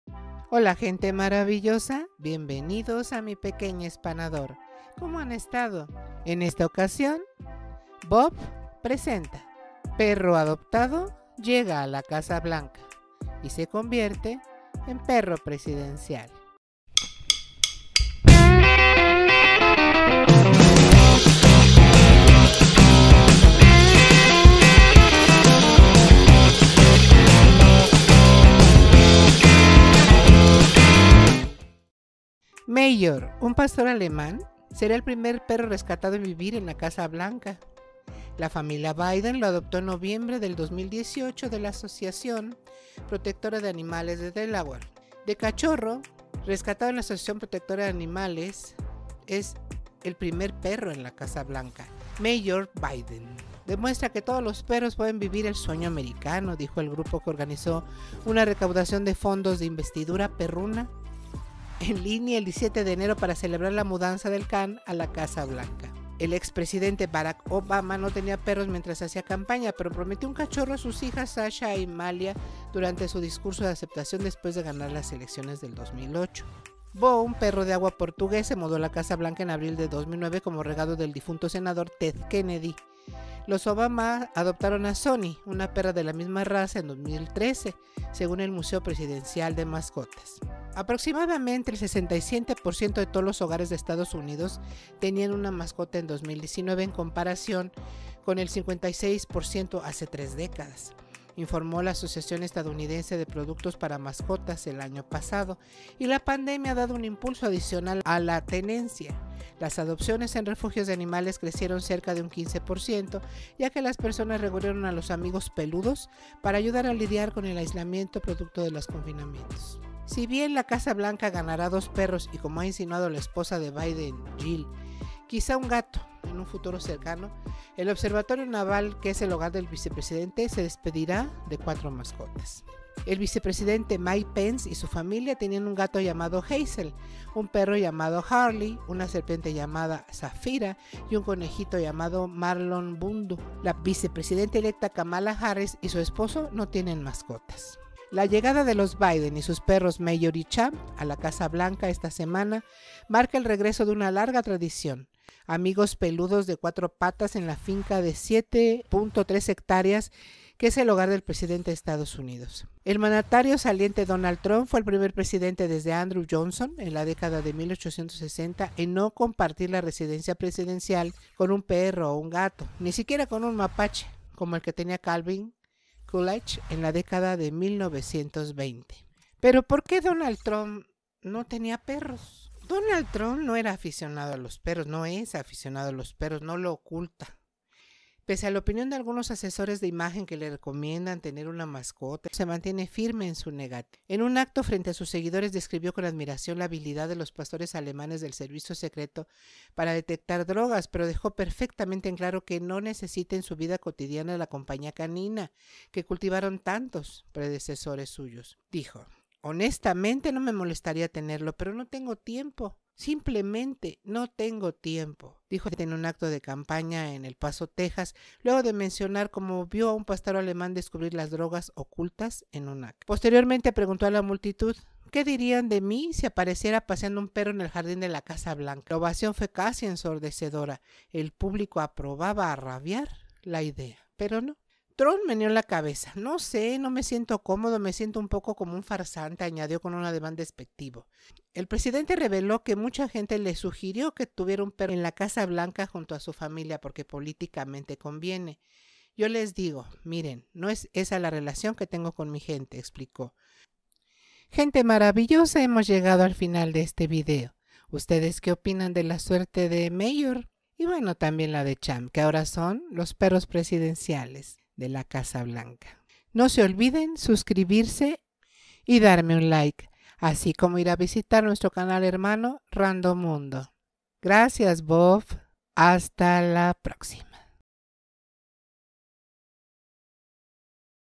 PERROS CASA BLANCA ¡QUE SUERTUDO! MAJOR, PERRO PASTOR ALEMAN ES ADOPTADO.
LOS PERROS DE BIDEN MAJOR Y CHAMP LLEGAN A LA CASA BLANCA